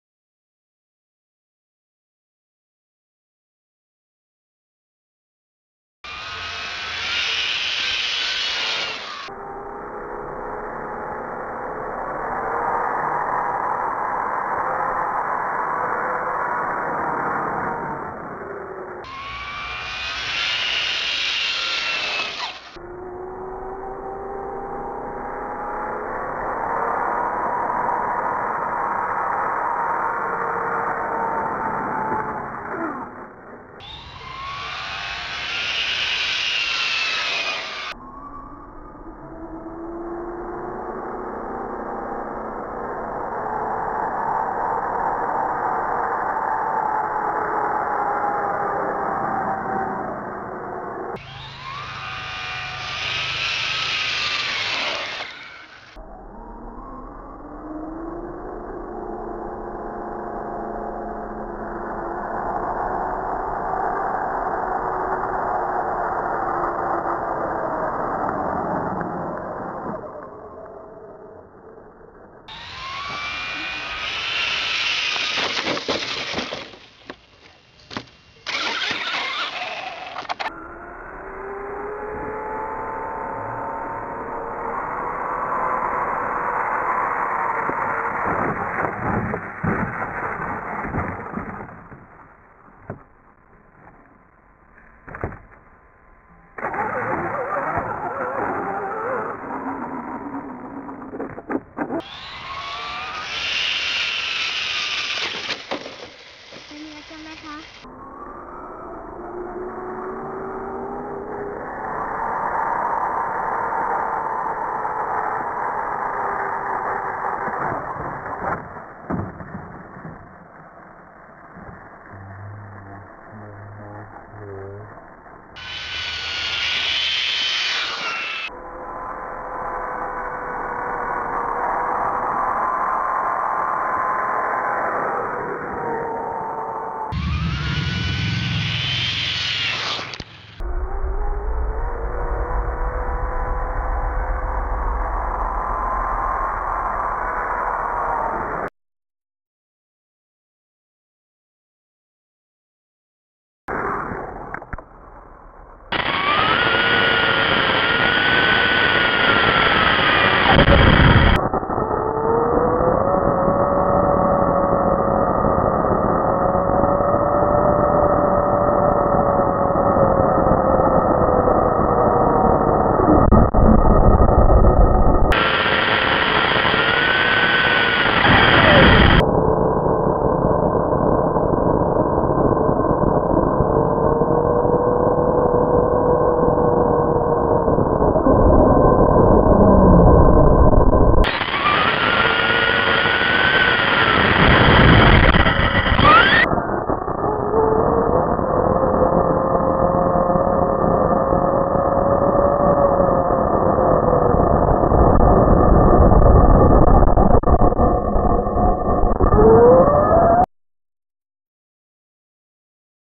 Traxxas Slash 4x4 sur 2 roues
Traxxas Slash 4x4 sur 2 roues, avec lipo 3s, pneus Steet Fighter, moteur Velineion VXL, pignon couronne du stock ! 2è partie de la vidéo avec caméra embarquée Epic's Cam